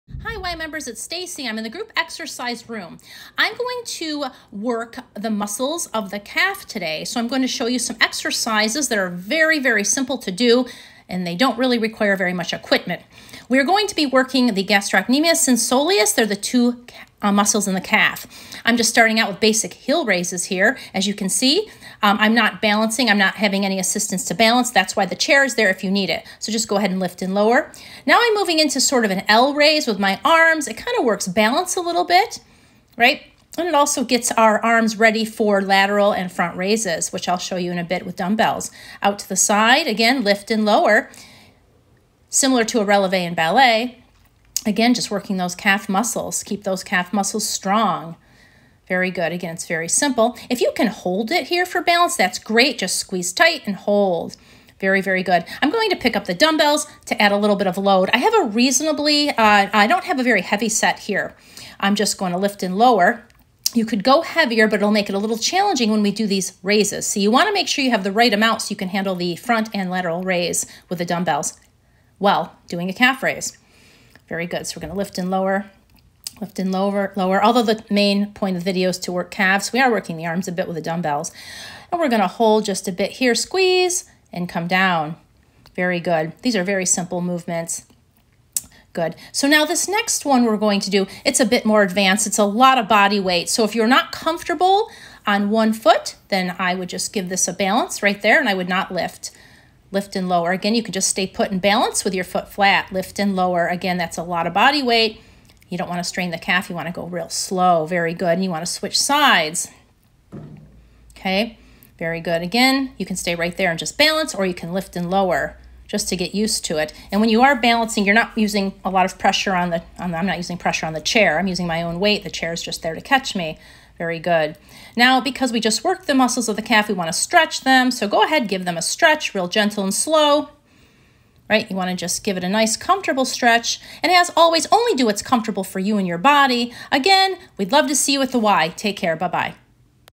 Calf Muscle Basics
Hi Y Members, Here are a few exercises to work your calf muscles. Progressions are demonstrated so only do what is comfortable for you. See you at the Y! :)